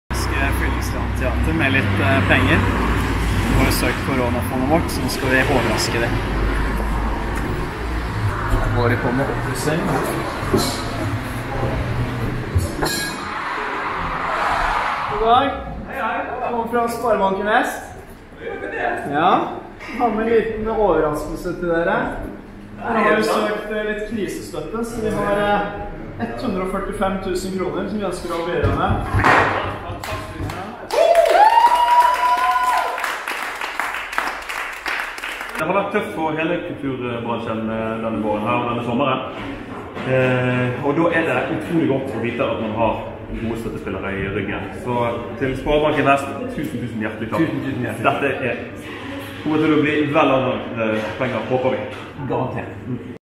FyllingsdalenTeater#2
Fyllingsdalen Teater får krisestøtte fra Korona-fondet